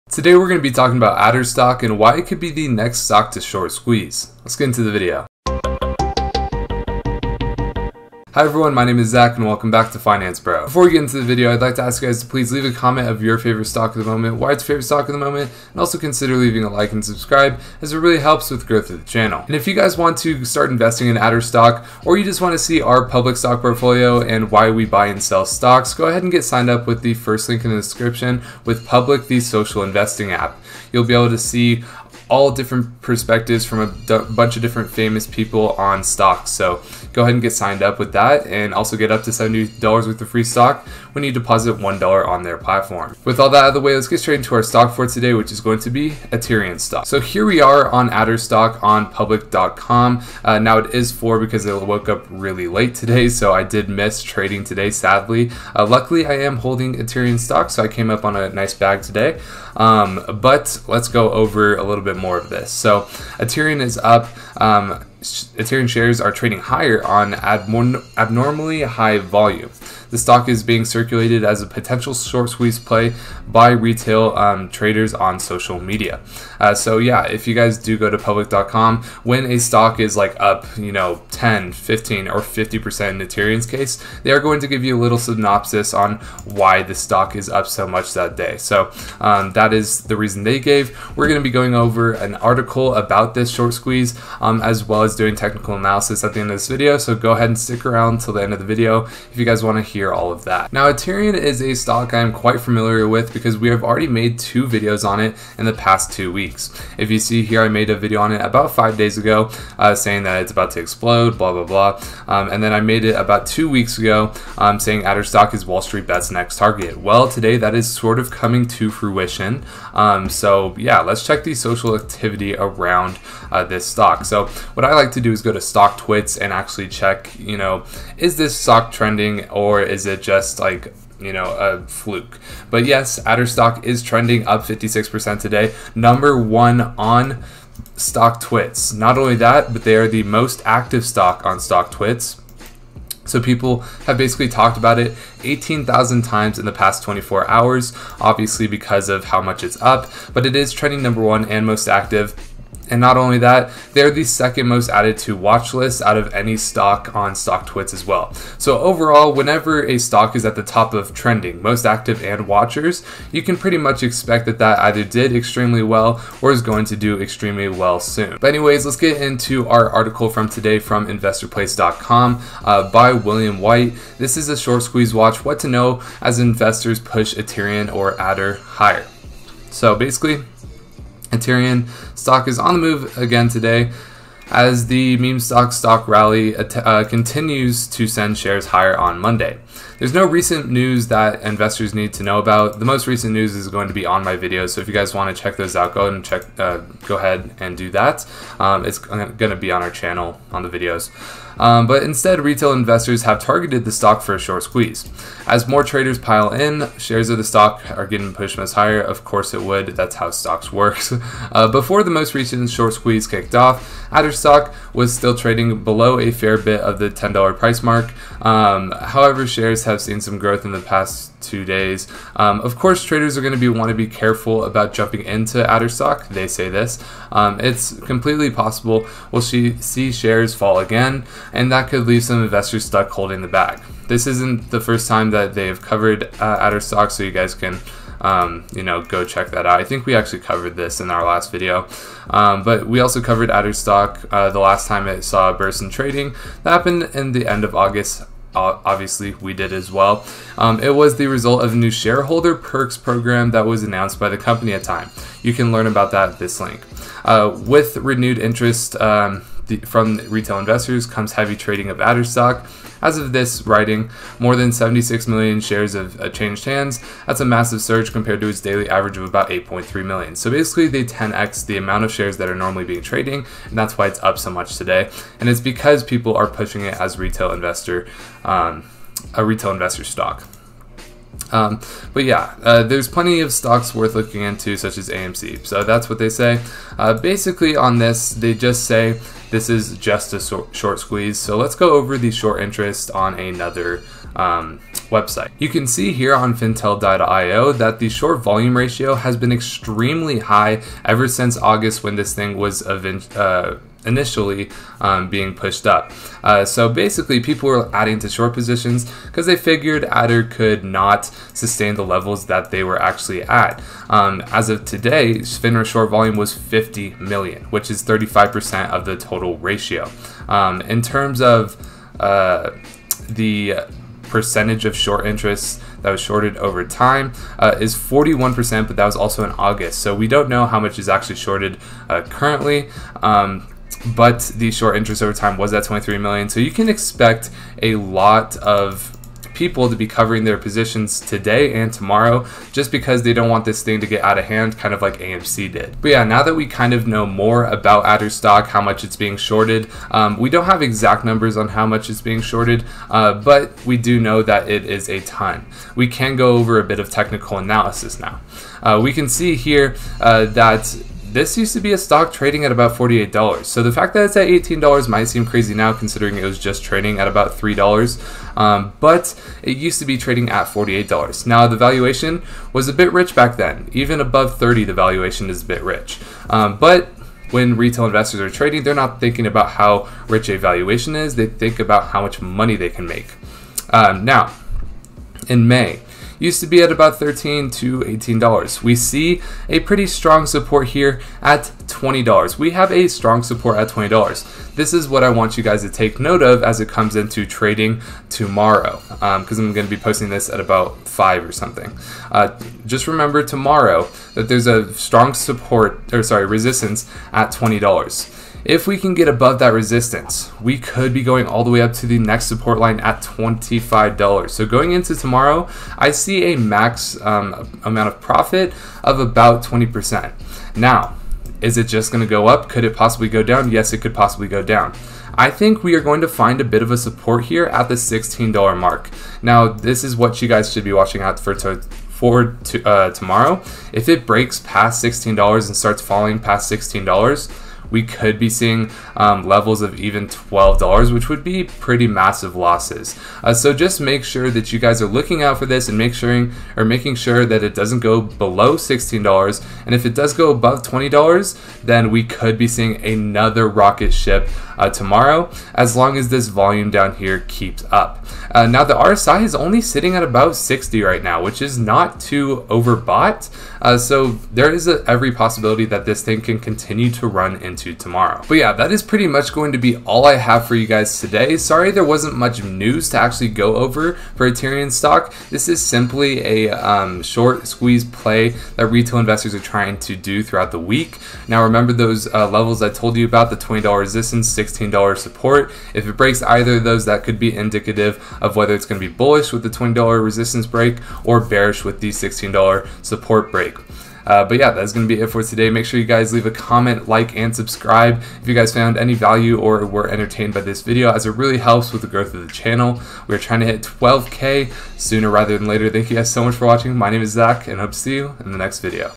ATER Stock Is About To SHORT SQUEEZE (Aterian Stock)
In this video, we go over ATER Stock or Aterian Stock and why it is going to short squeeze.